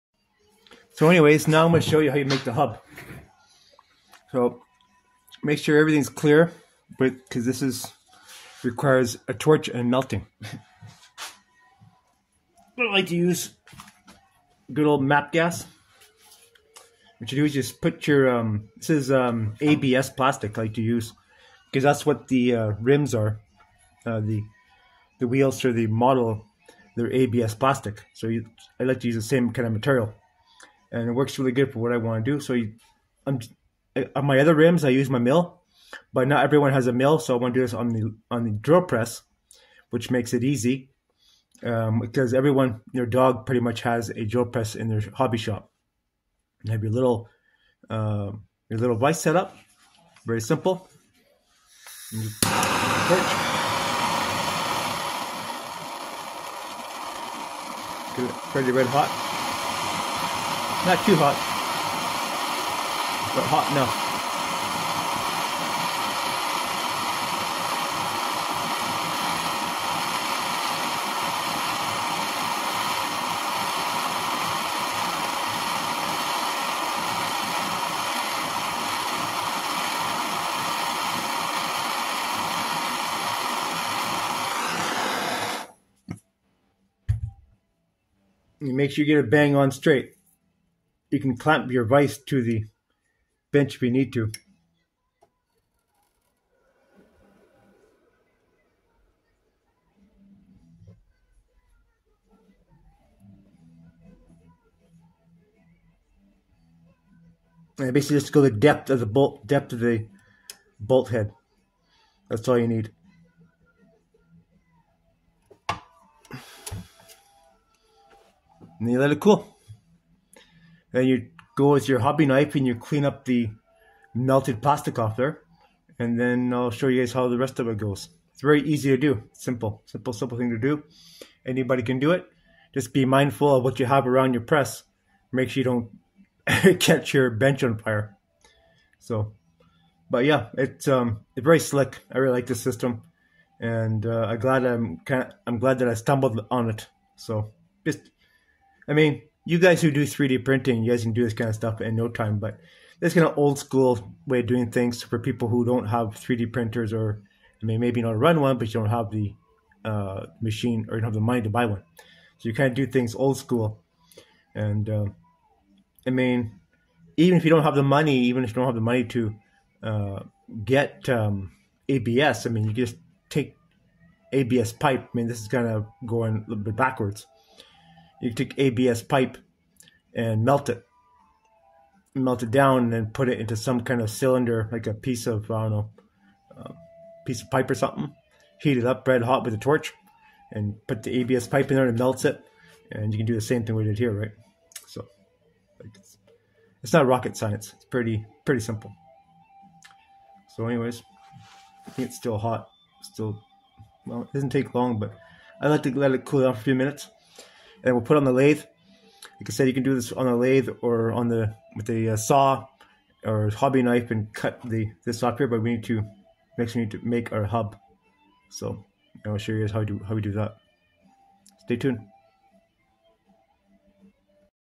How to make wheel hubs for rc trucks in abs plastic 3
Here is how you melt in the hex bolt pattern in you abs plastic hub . I use my milling machine to make my hubs because it more precision on measurements .But for this video I'm using a basic drill press which anyone can afford to buy or has one already in there shop .